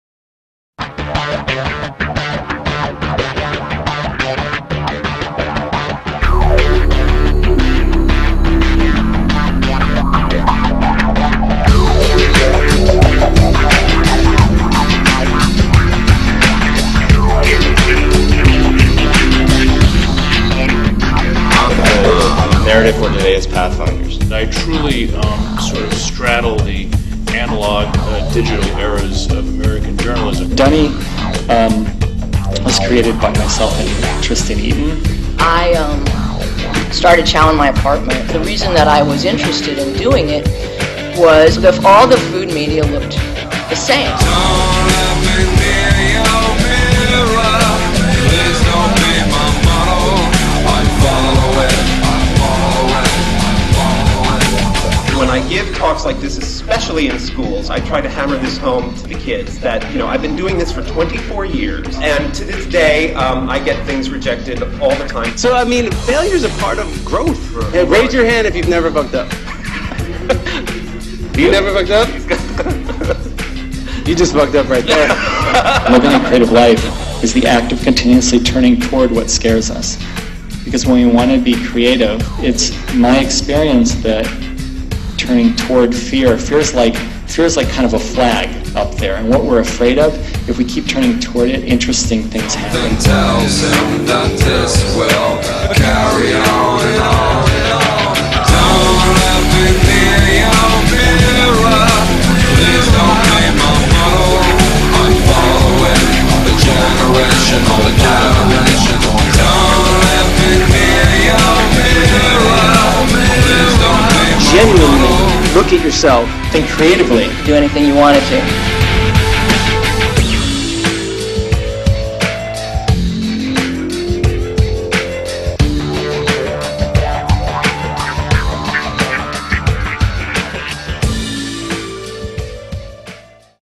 Imprint Culture Lab - NY 2009
An afternoon speaker series featuring the pathfinders that have brought us new acumen in design, cuisine, entrepreneurship and lifestyle branding.